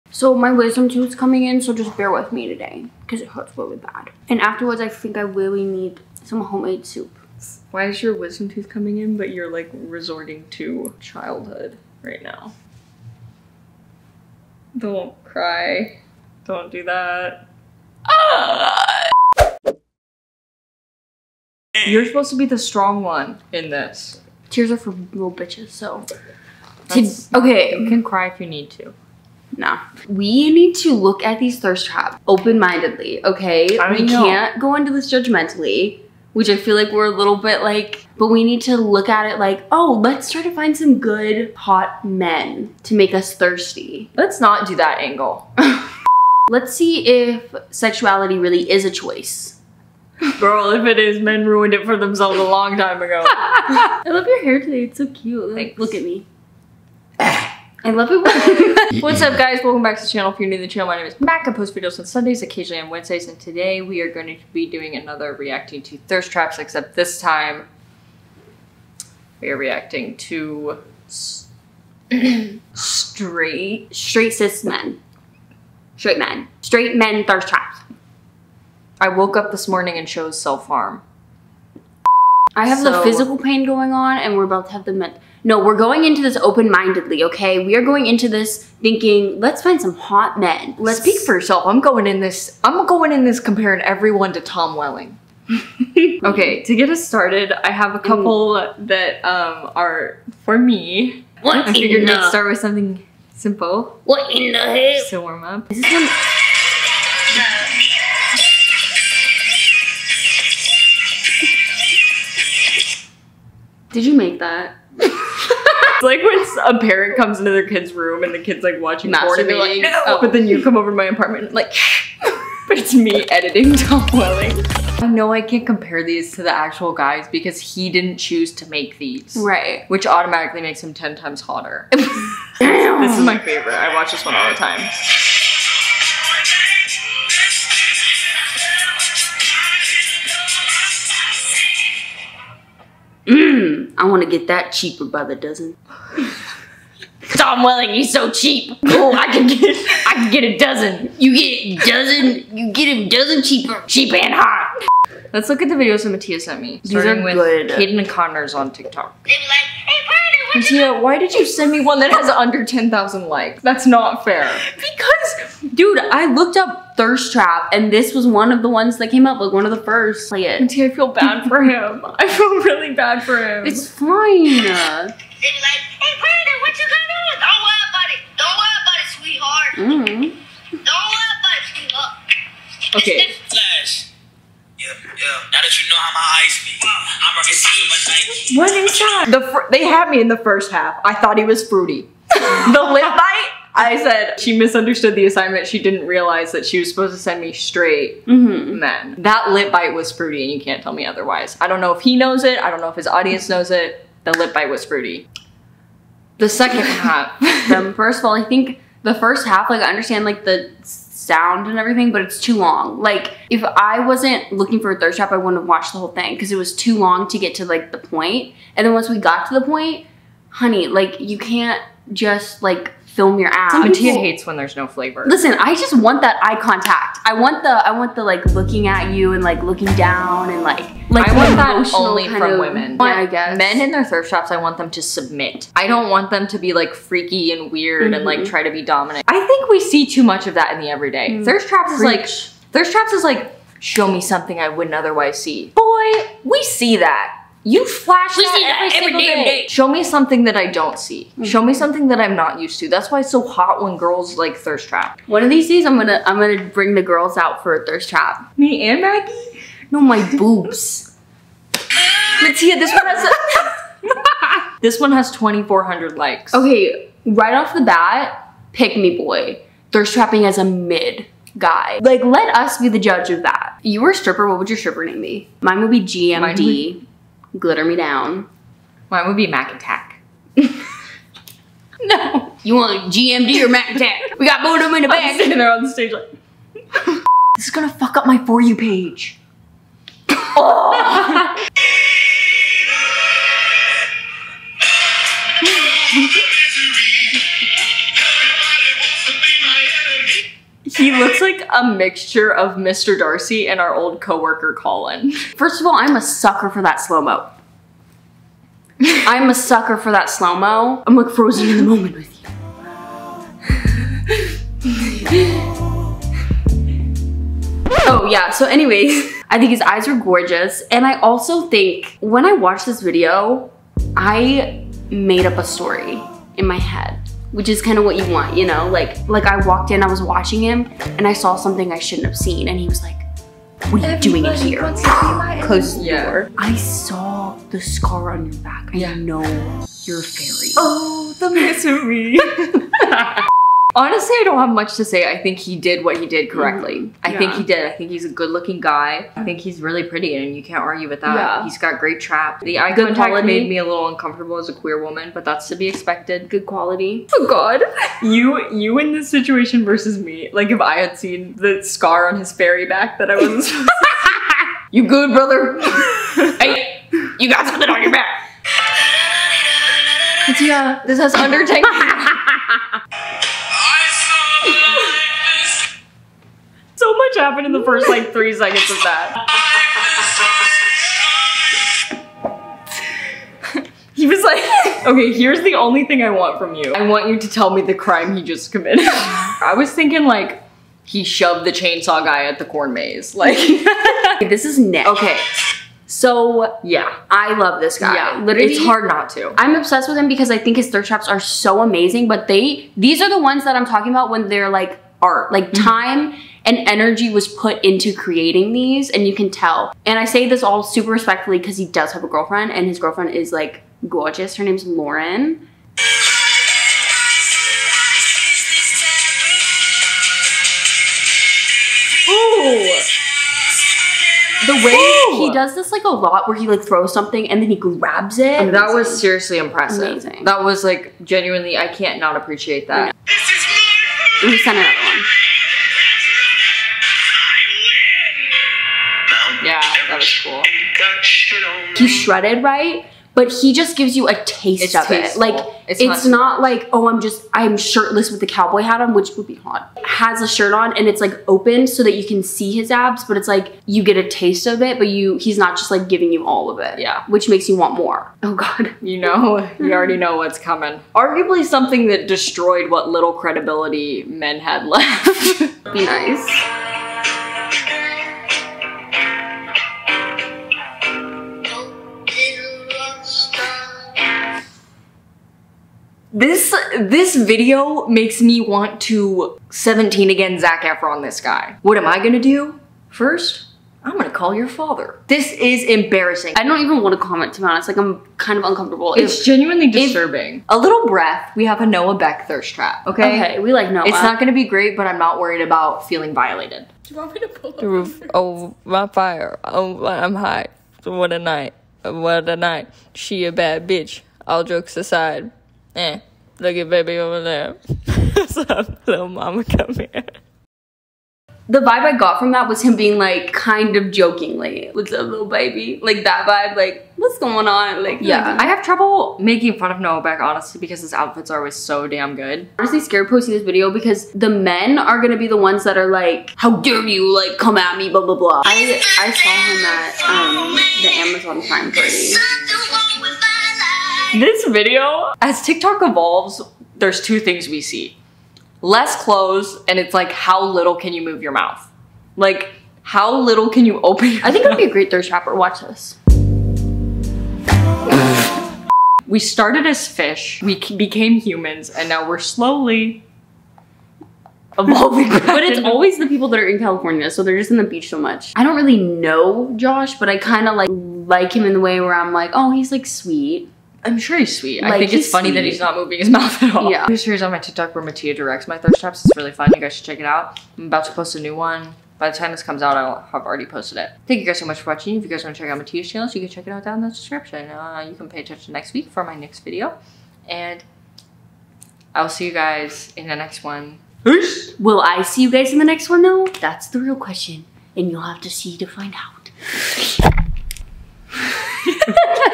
REACTING TO STRAIGHT BOY THIRST TRAPS ft. @matteaing (never again.)
women are superior. hope you all have a great week, sorry the video is a day late.
love always,
mak